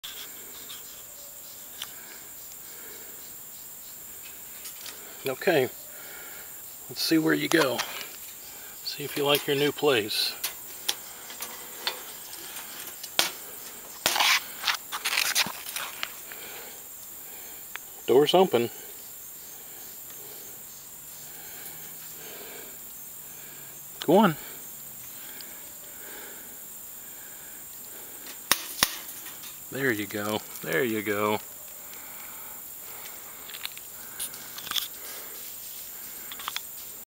Groundhog Release
This guy (or gal) decided to set up shop in our basement and did not get the subtle hints that we didn't want a houseguest... so he got evicted. Though, I have to assume he likes the new diggs a lot better... this is a heavily wooded natural preservation with streams and a lovely greenway goes through it, so we can visit him!